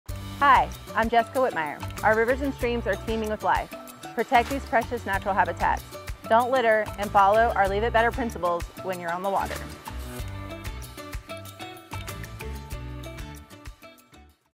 Help to "Leave it Better" - Jessica Whitmire of Headwaters Outfitters (Video Two)
One of our local outdoor recreation experts, Jessica Whitmire of Headwaters Outfitters, has a quick tip for how you can “Leave It Better” when enjoying Transylvania County’s rivers and streams. Find out more at Explorebrevard.com/leave-it-better/.